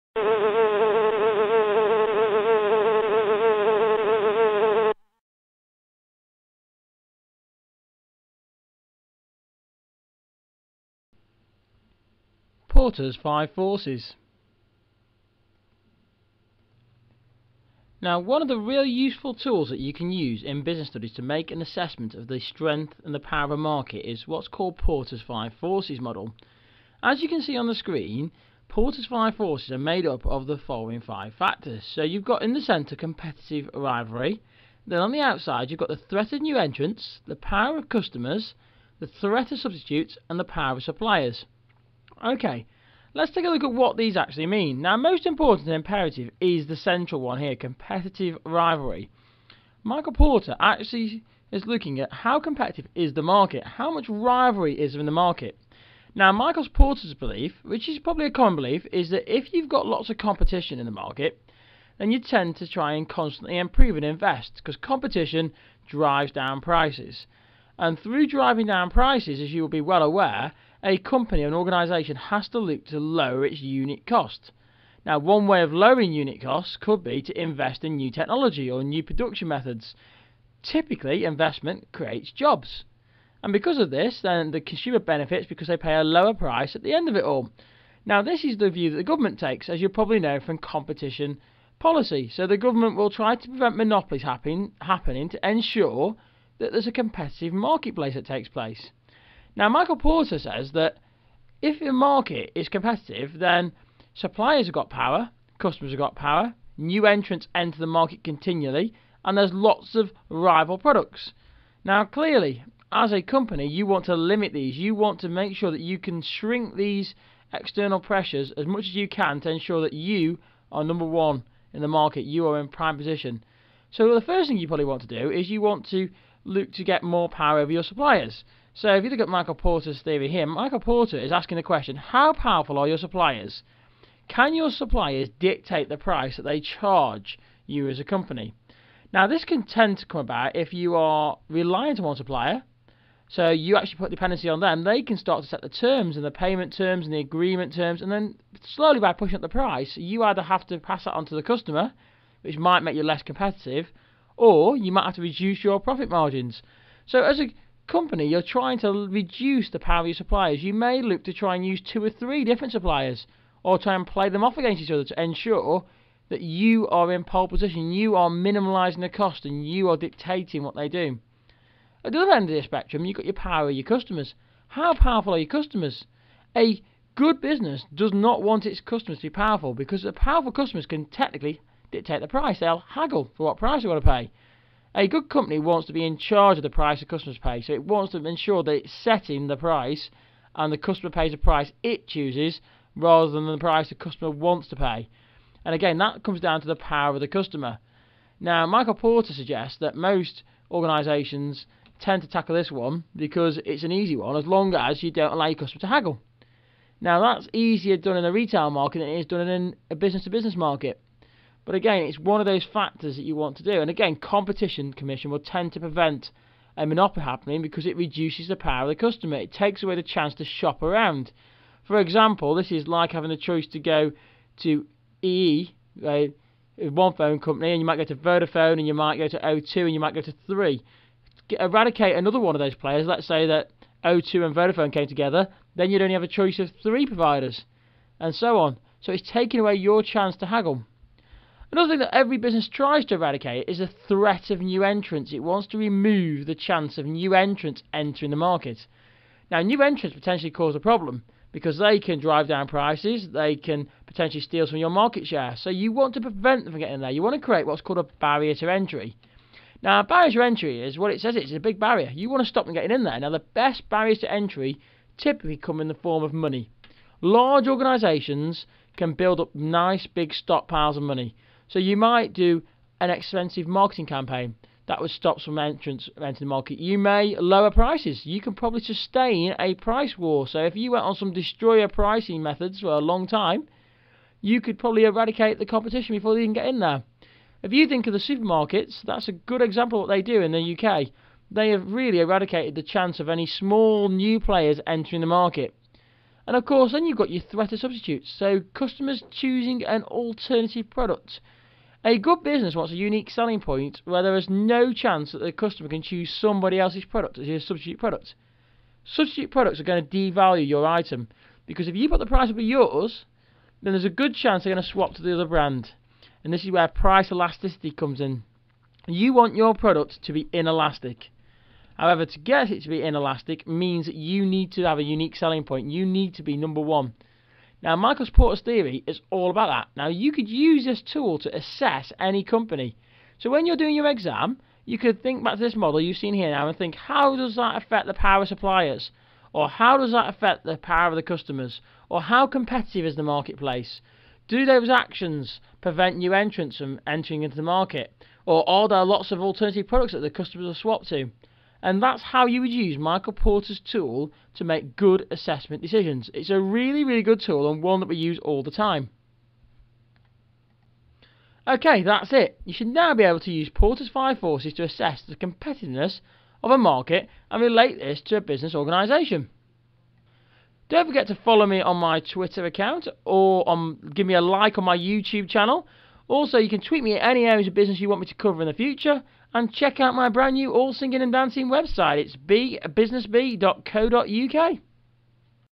Porters 5 Forces Tutorial
Here BeeBusinessBee looks at the concept of Michel Porter and his 5 Forces model. Each part of the model is investigated in some detail before looking at how you can use this model in your Business Studies exams, such as BUSS4.